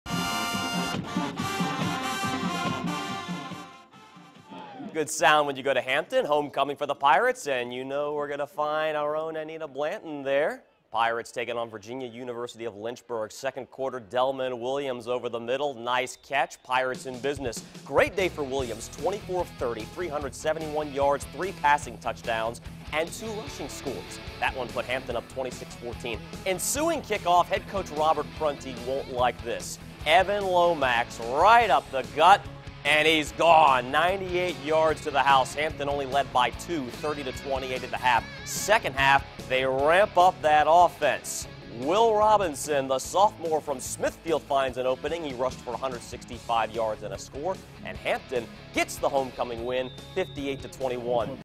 Hampton rolls past VA-Lynchburg on Homecoming 58-21
Pirates quarterback Delmon Williams three three touchdowns and rushed for two more to lead his team past an upset-minded Virginia University of Lynchburg.